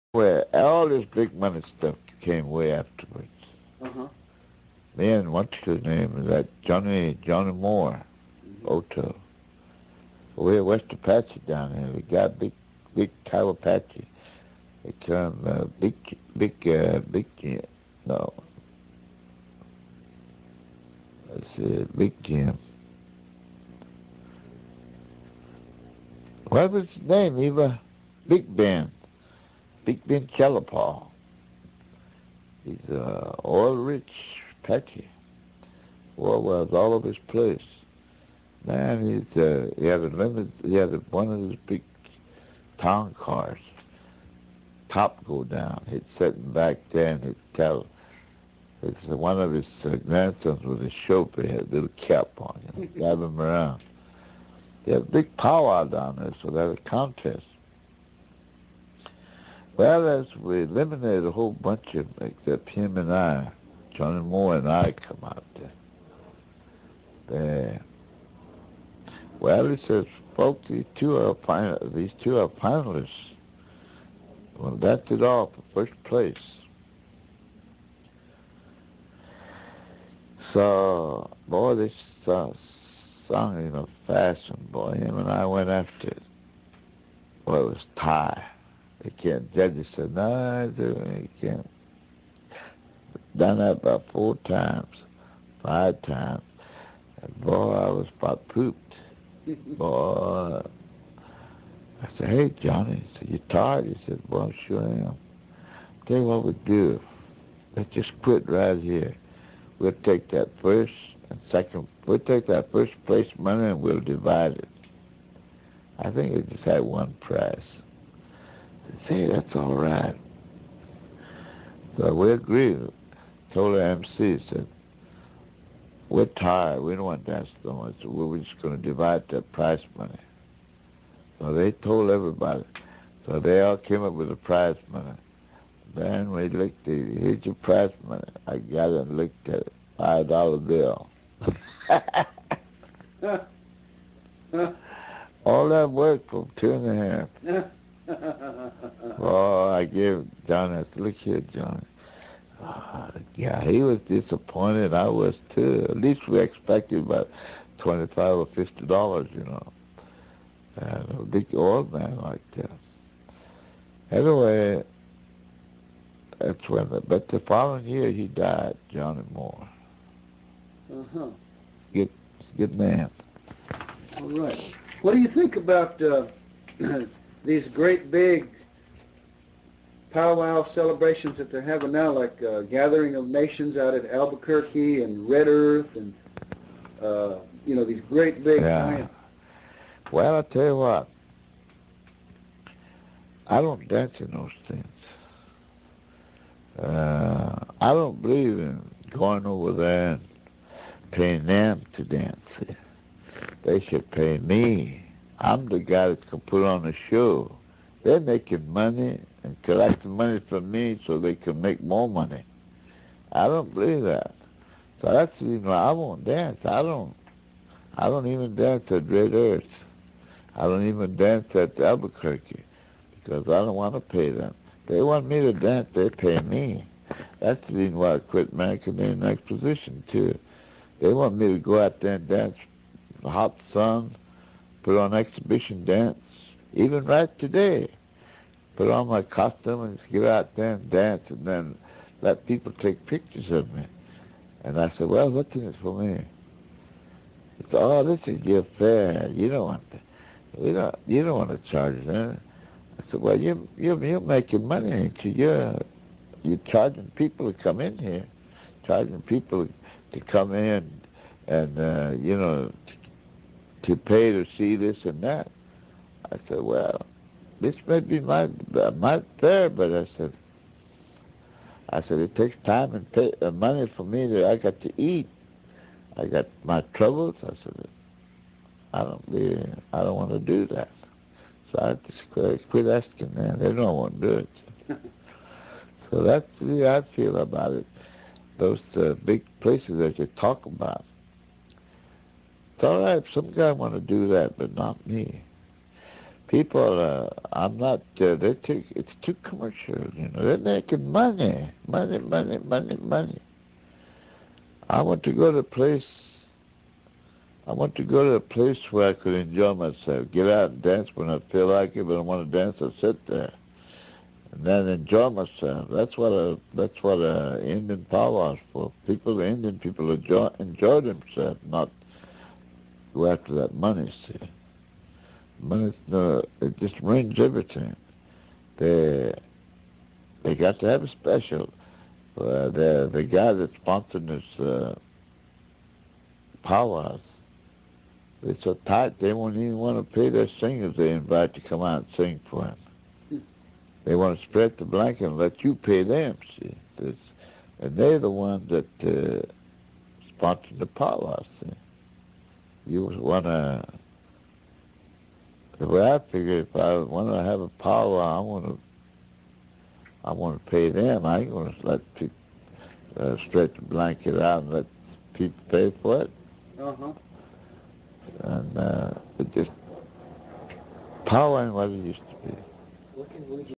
Woogie Watchetaker, Prize Money & Big Pow-wows -- Part IV.wmv
"Woogie" Watchetaker, Prize Money & Big Pow-wows -- Part VI

George Smith "Woogie" Watchetaker relating an anecdote about receiving prize money and his opinion regarding big extravaganza contest pow-wows.

The late great "Woogie" Watchetaker was Comanche and the world's greatest Southern Fancy Wardance Champion. He had a distinguished career as a Southern Fancy Wardancer and was known nation-wide thoughout the Native American communities as a champion dancer. Woogie performed before the Queen of England and was said to have been knighted "Lord George" by the Queen of England. He was a traditional Native American painter and a spiritual leader for the Comanche people.

George Smith "Woogie" Watchetaker, March 1, 1916 -- May 27, 1993.

Excerpted from raw footage taken by Scott Swearingen. Circa 1990's.

Interviewer: Sandy Rhoades. Producer/Videographer: Scott Swearingen. Production Company: Full Circle Videos.

(Video & audio becomes out of sync at the end of the video. Don't have the expertise to correct this problem. Sorry.)